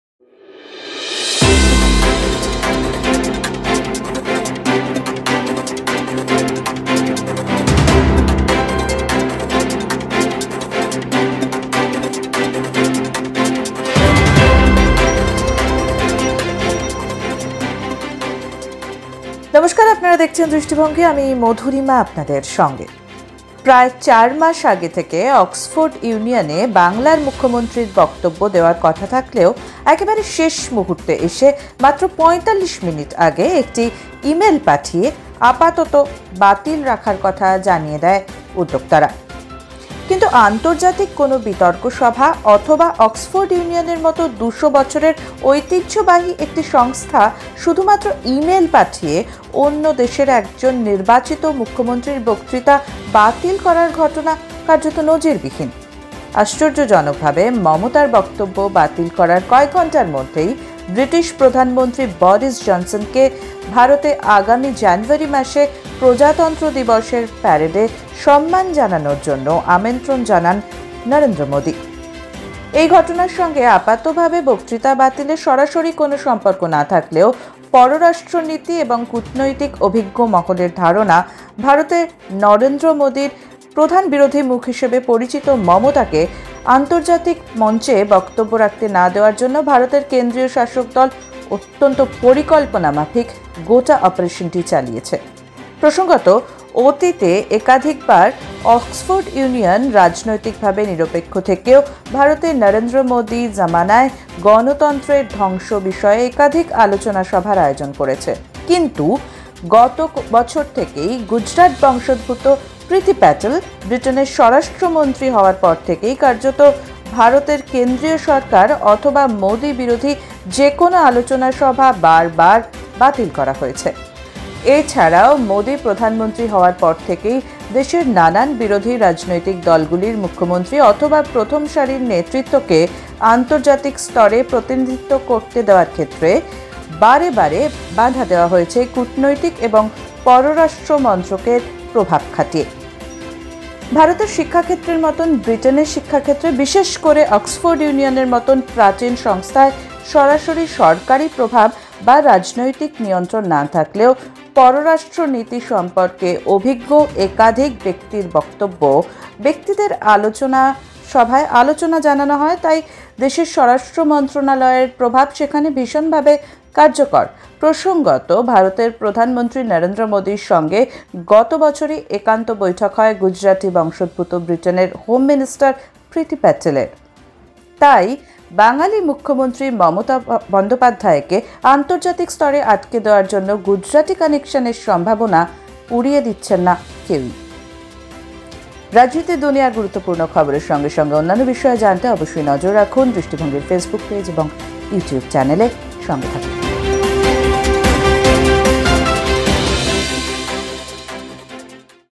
অক্সফোর্ডে বলতে দেওয়া হল না মমতাকে, নেপথ্যে পিএমও? | Drishtibhongi | দৃষ্টিভঙ্গি
Breaking News Music Credit: Music for Video Library